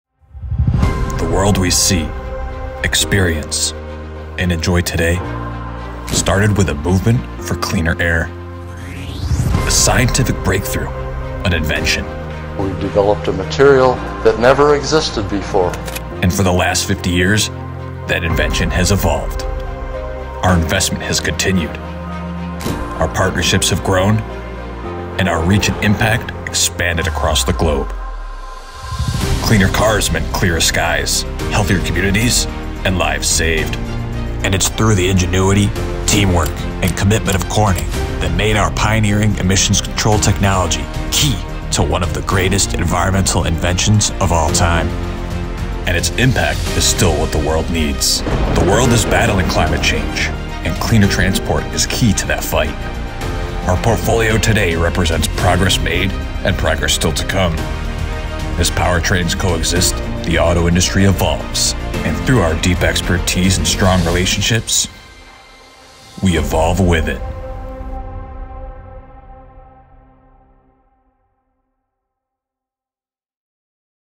Celebrating 50 years of Environmental Technologies
Over the past 50 years, Corning’s ceramic substrates and particulate filters for emissions control have drastically reduced vehicle-related pollution in cars, trucks and equipment and contributed to significant improvements in air quality around the world.

According to the Environmental Protection Agency, these technologies have helped save hundreds of thousands of lives each year in the U.S. alone. Emissions control technologies continue to play a vital role in Corning’s business portfolio and in the automotive industry’s ongoing efforts to reduce air pollution and combat climate change.